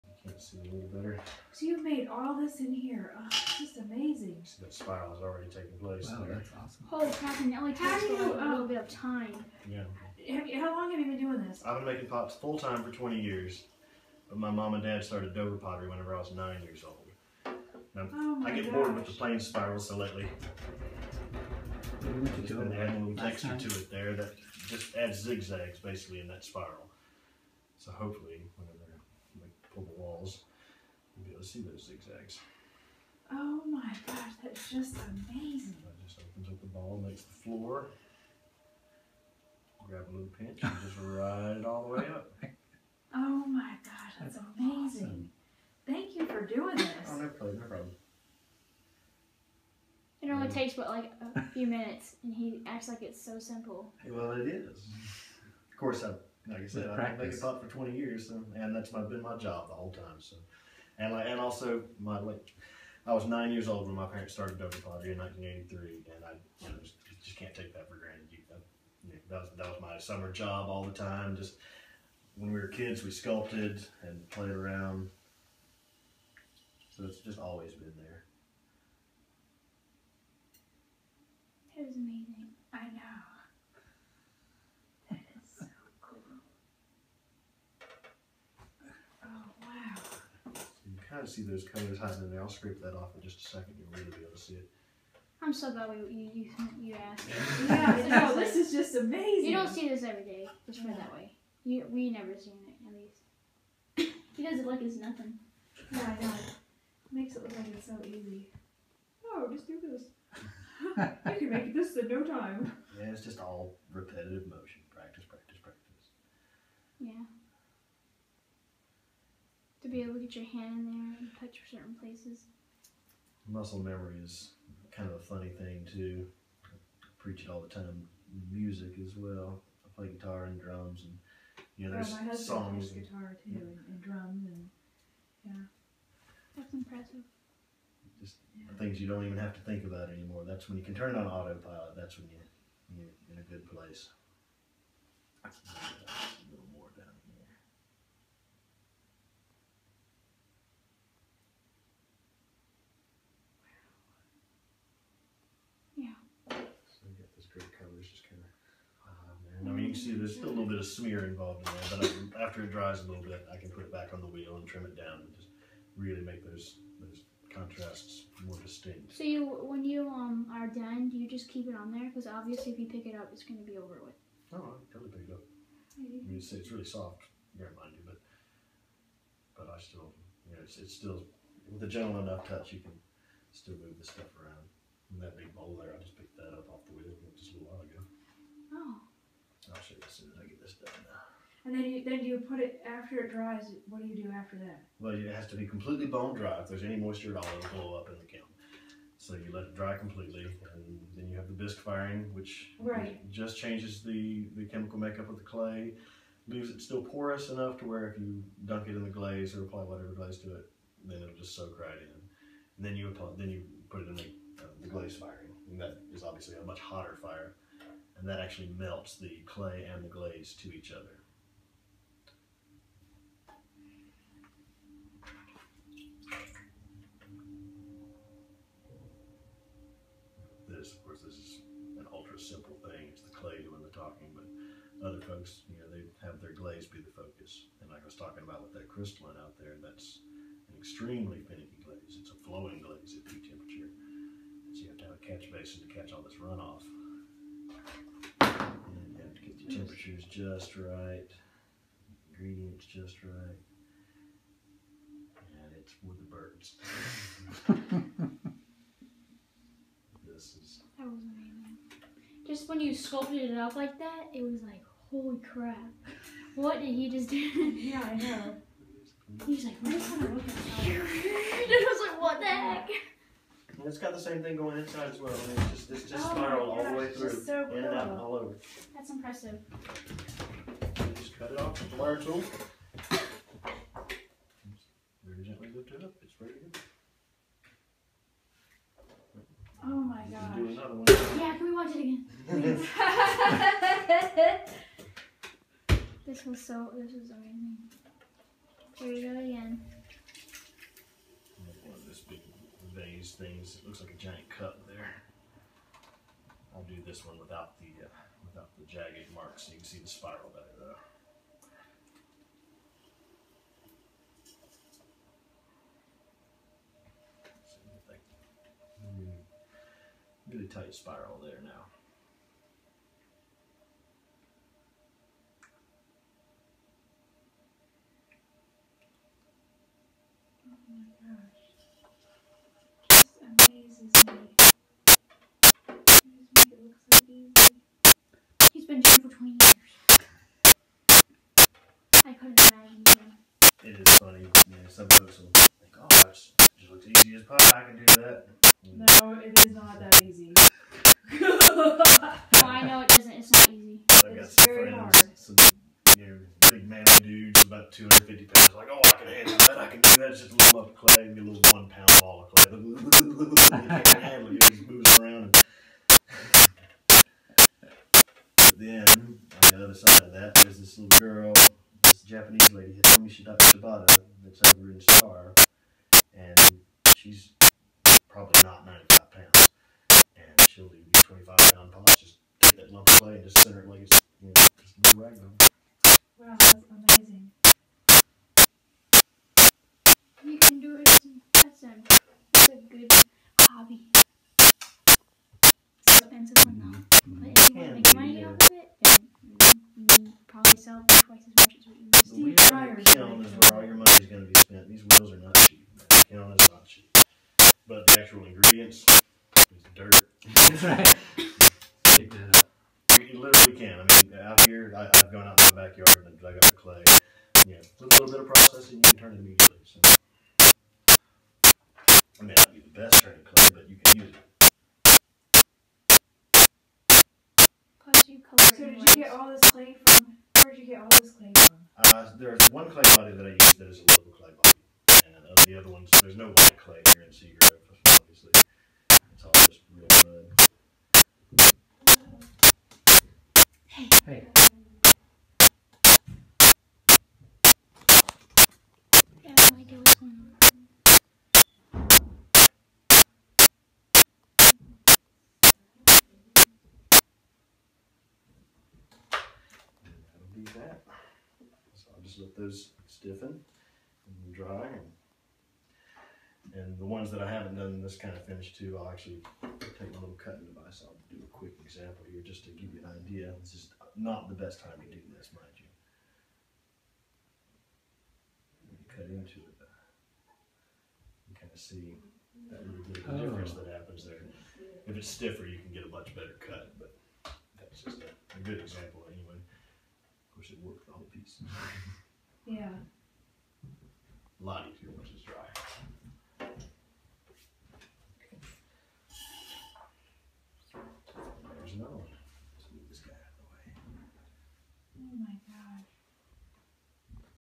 Eck McCanless Pottery demonstration creating two pots
Pottery turning demonstration by Eck McCanless in Seagrove, North Carolina.